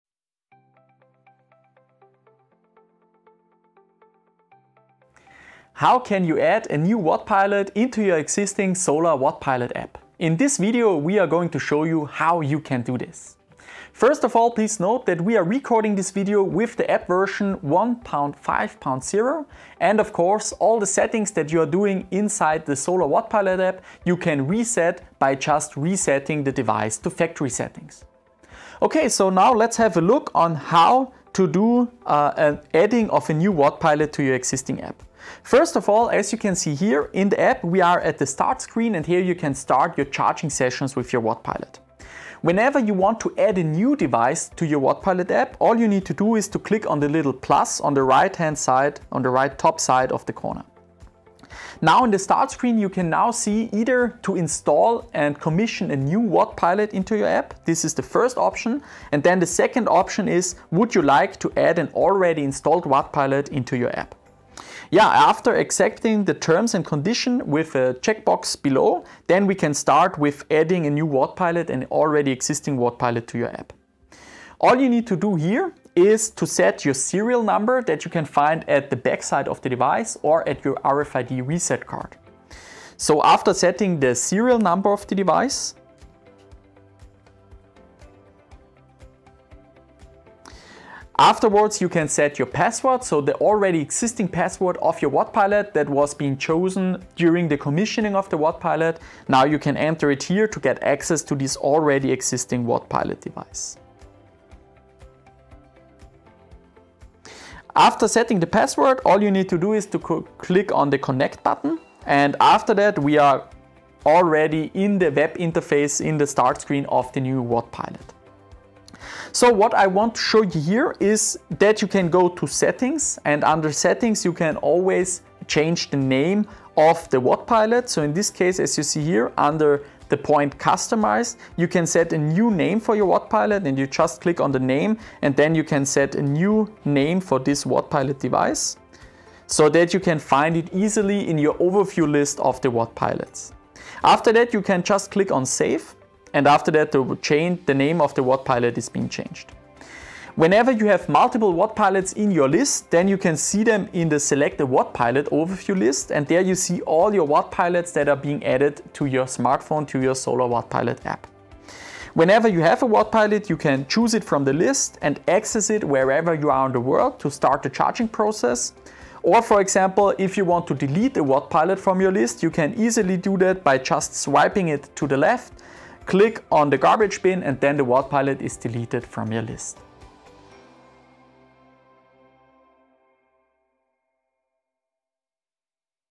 How-To Video: Add new Wattpilot in the Solar.wattpilot App
📃 VIDEO INFOS
In this how-to video, we show you how quickly and easily you can add a new Fronius Wattpilot in the Solar.wattpilot app.

All questions about the further setting of all functions and features can be found in this webinar